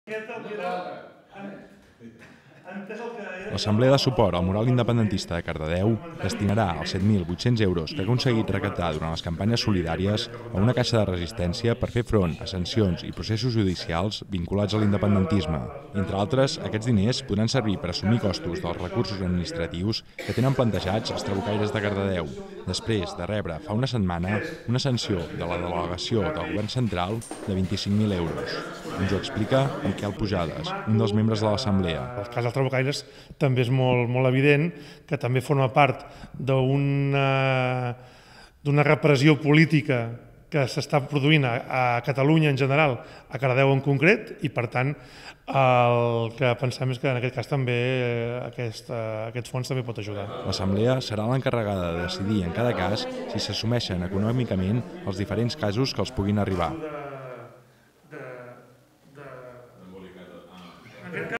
L'assemblea del "Jo també hi era" destinarà els diners a una caixa de resistència
Aquest dimecres a la nit l'assemblea del mural independentista va reunir una vintena de persones per decidir que fer amb els diners recaptats desprès que el passat 8 d'octubre els 8 encausats fossin completament absolts per l'Audiència Provincial.